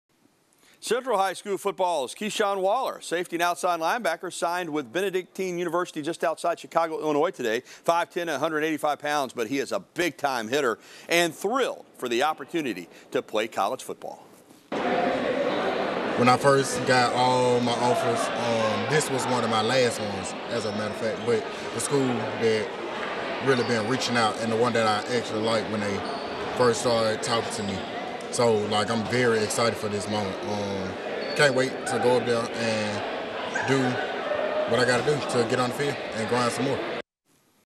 Central High School's Keyshawn Waller signs with Benedictine University
Keyshawn Waller out of Central High School signed to go play football for Benedictine University.

Waller is 5- foot-10 and 185 pounds. He played linebacker and safety in high school.

Benedictine University was one of the last schools to offer Waller but how much they communicated with had a big impact on his decision.

“When I first got all of my offers, this was my last one as a matter of fact, but they’re the school that’s really been reaching out and one that I actually liked when they first started talking to me so I’m very excited for this moment,” Waller said.

Benedictine University is located just outside of Chicago, Illinois. The Eagles compete in the Northern Athletics Collegiate Conference (NACC) at the NCAA Division III level.